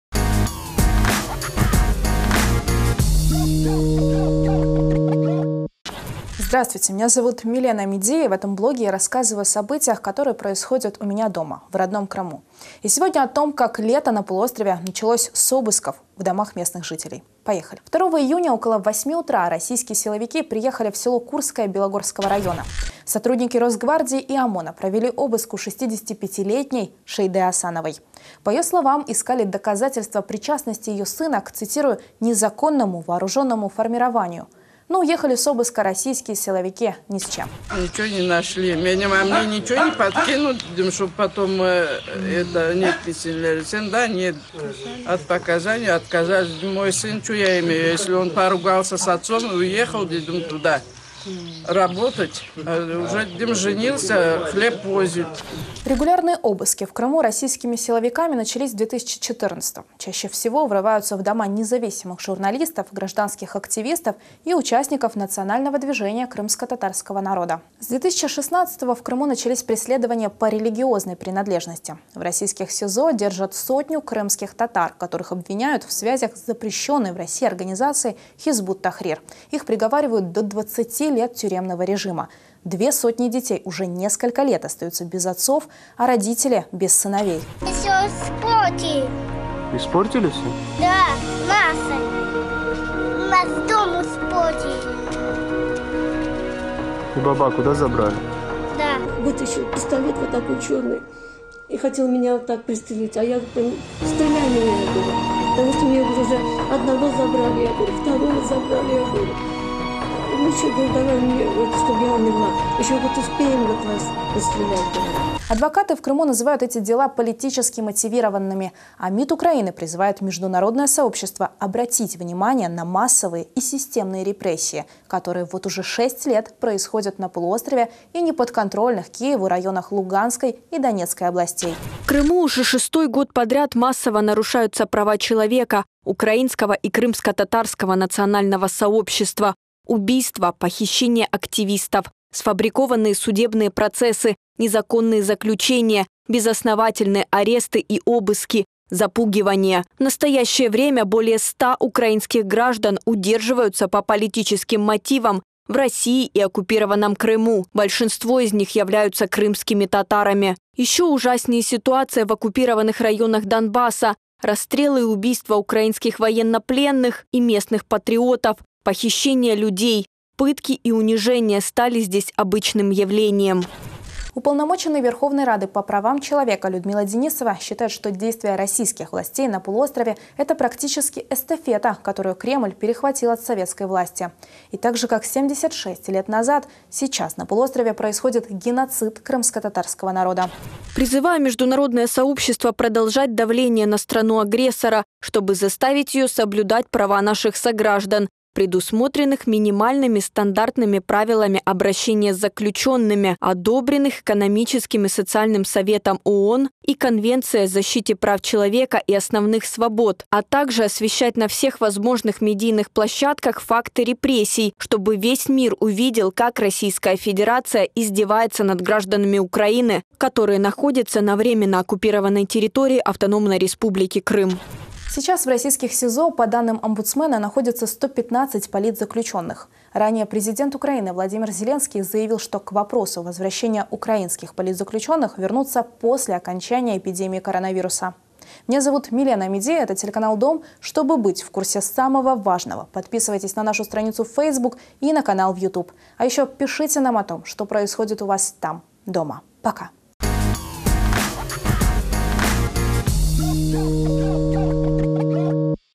Незаконные обыски и аресты в Крыму | А как там дома?
Регулярные обыски в домах местных жителей Крыма силовиками РФ начались в 2014-м. Чаще всего врываются в дома независимых журналистов, гражданских активистов и участников национального движения крымскотатарского народа. Один из недавних инцидентов случился 2 июня. Около 8 утра российские силовики приехали в село Курское Белогорского района и провели обыск у 65-летней Шейде Асановой. По словам женщины, они искали доказательства причастности ее сына к "незаконному вооруженному формированию". Подробнее о ситуации смотрите в блоге журналистки Милены Амеди.